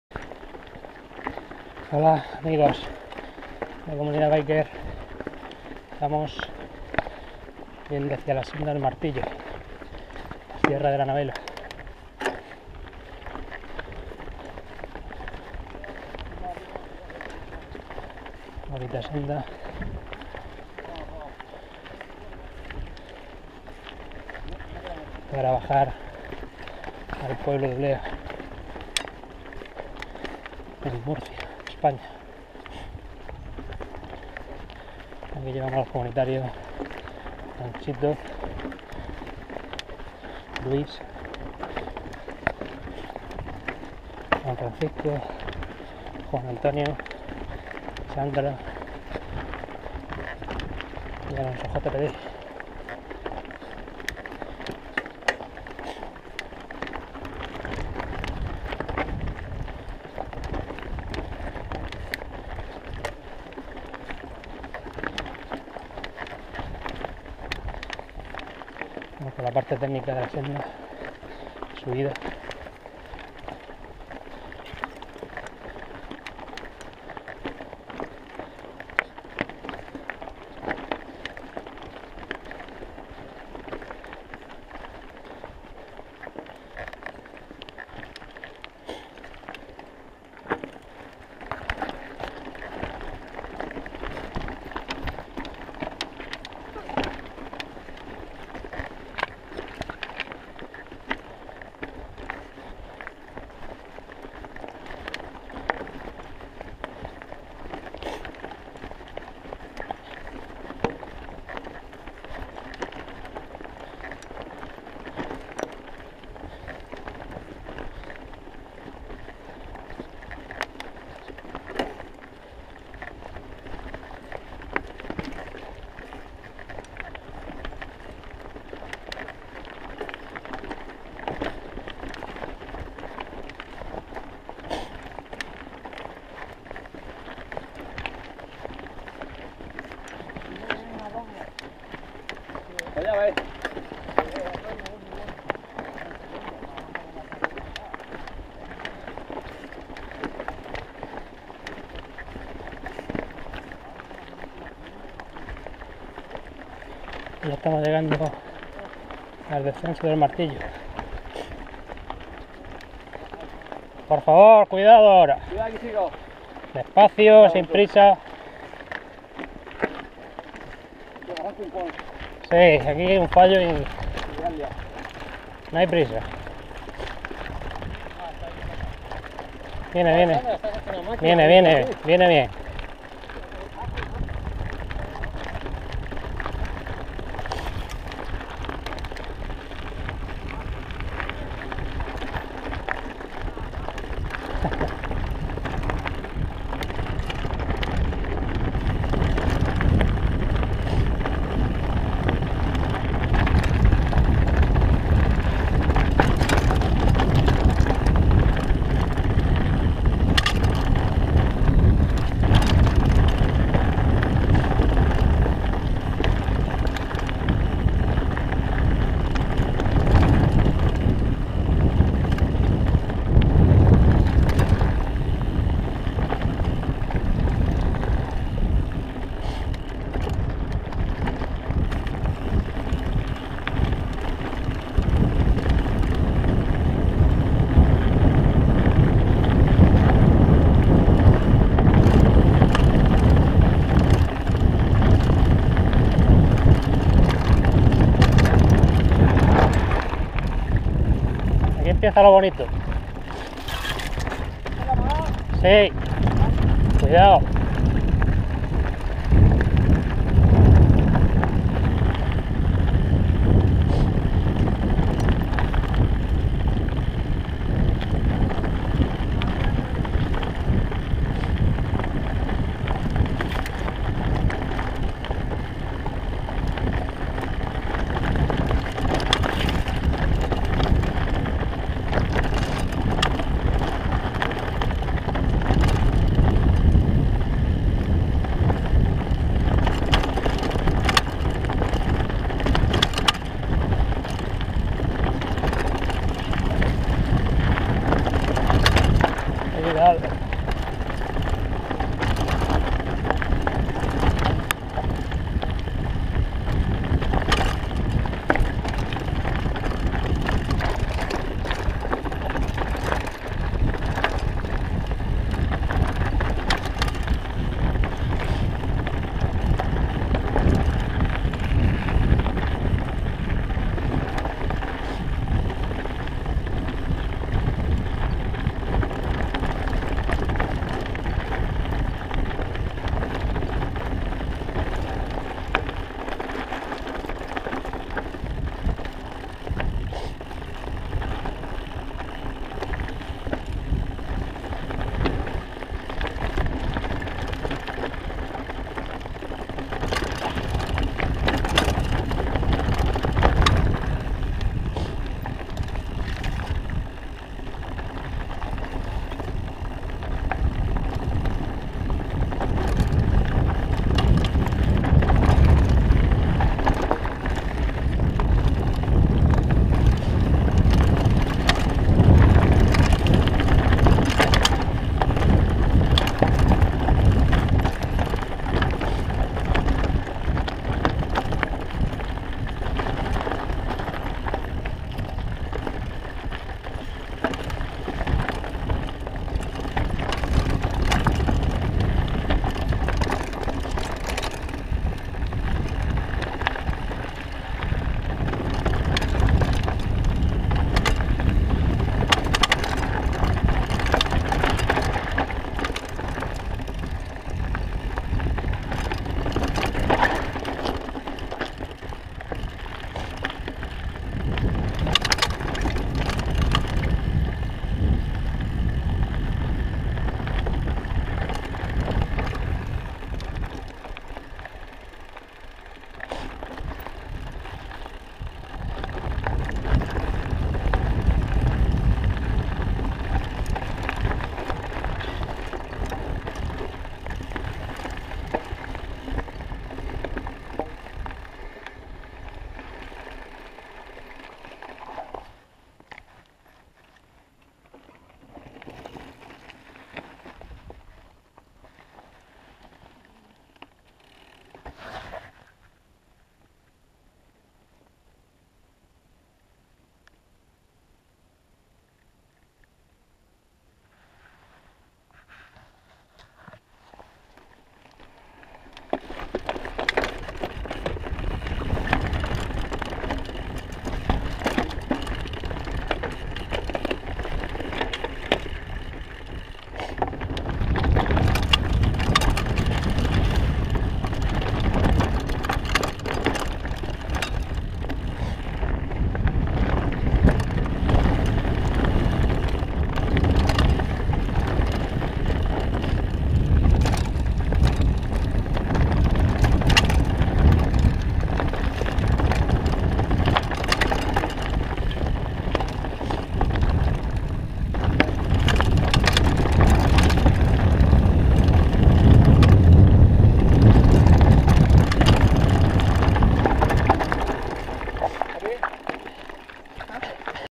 MTB senda Martillo en Sierra Navela descenso MTB BTT
Ciclismo de montaña por la senda del Martillo en la sierra de la Navela, entre Ojós, Ulea y Blanca, en la Región de Murcia (España). Los comunitarios de Comunidad Biker MTB Alonsojpd, Kronxito, Luis, Juan Francisco, Juan Antonio y Sandra realizan una ruta de ciclismo de montaña (mountain biking, MTB, BT, BTT, XC, cross country cycling, enduro MTB, all mountain, downhill, DH) por la zona de la sierra de la Navela, junto a la sierra de Ricote. En este caso ascendemos y descendemos por la senda del Martillo, con unas preciosas vistas de la montaña